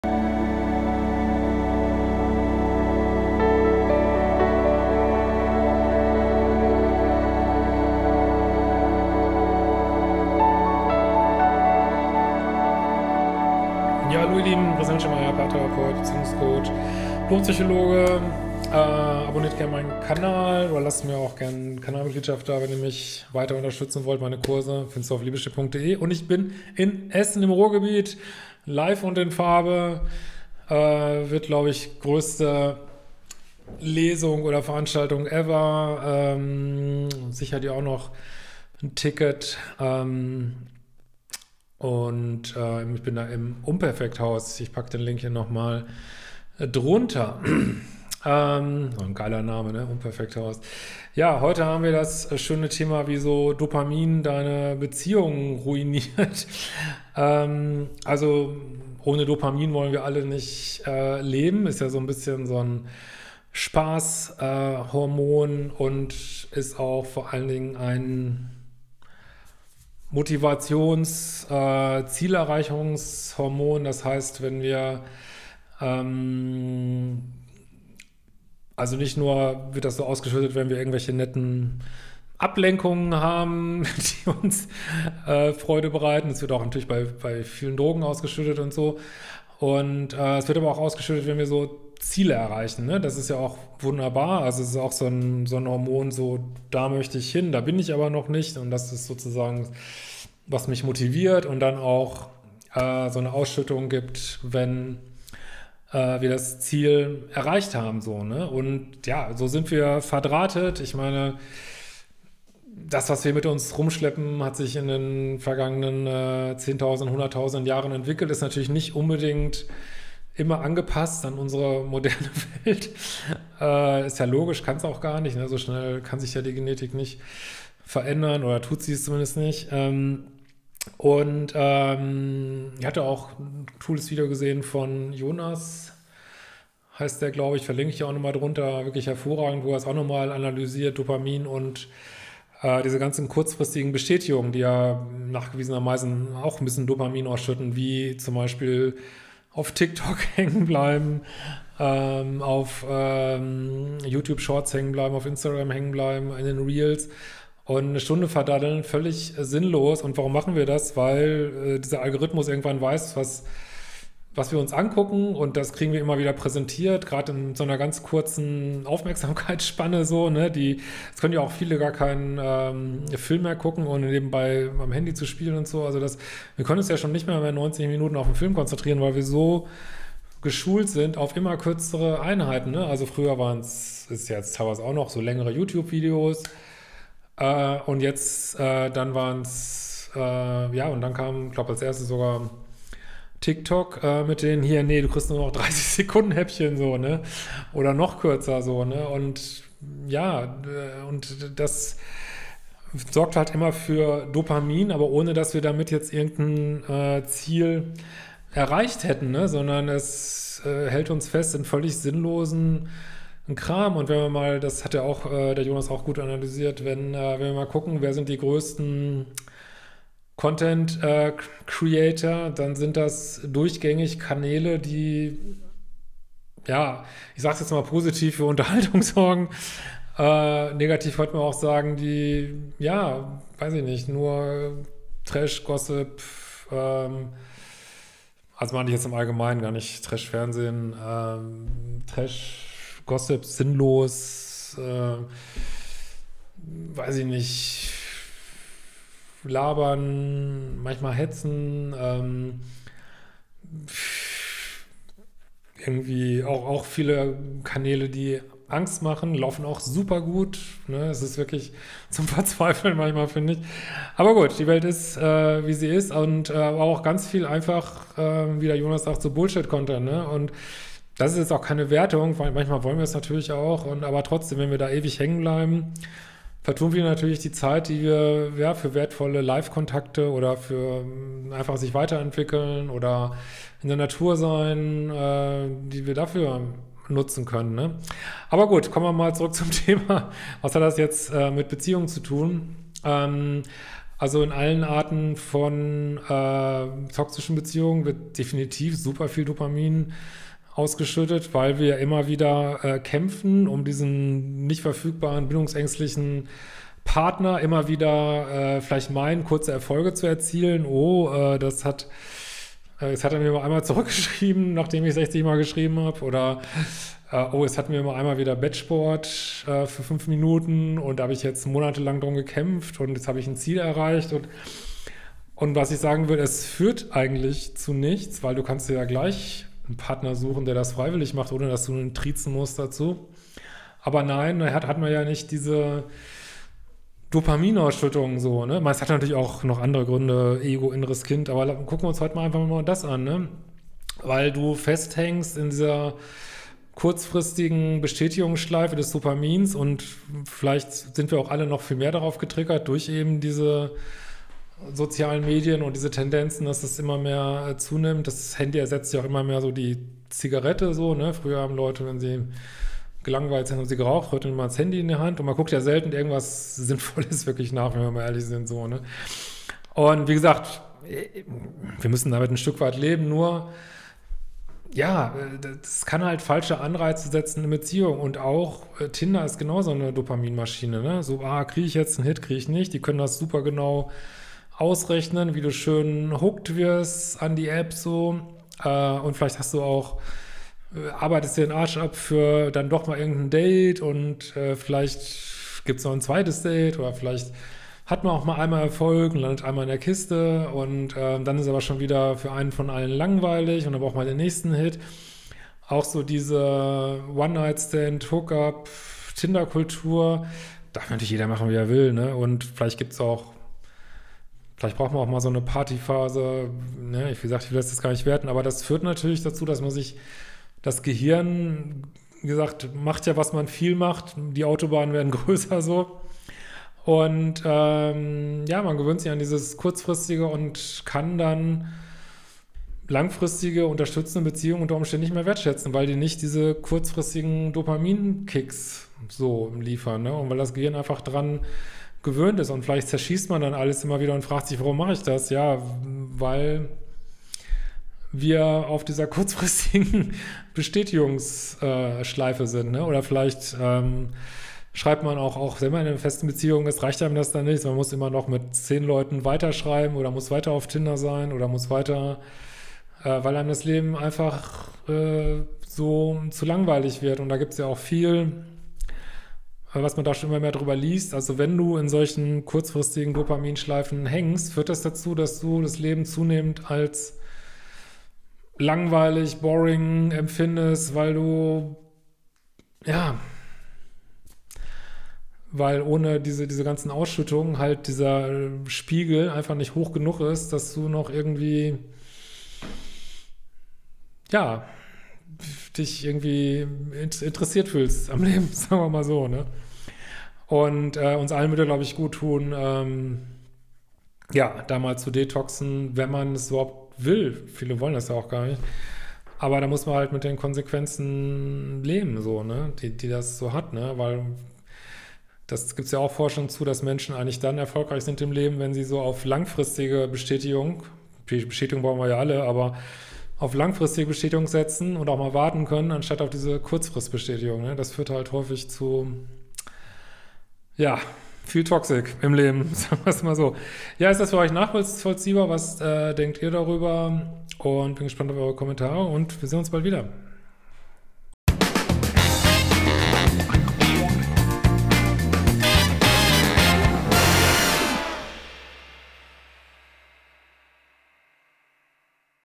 Warum Dopamin dein Beziehungsleben ruinieren kann
Korrektur: Dopamin ist natürlich ein Neurotransmitter und kein Hormon

Wir leben in einer Zeit, wo wir uns fast nur noch kurzfristige "Kicks" besorgen, auch viel zB im Bereich Social Media. Diese Gewohnheiten können auch die Fähigkeit zu langfristigen Beziehungen betreffen.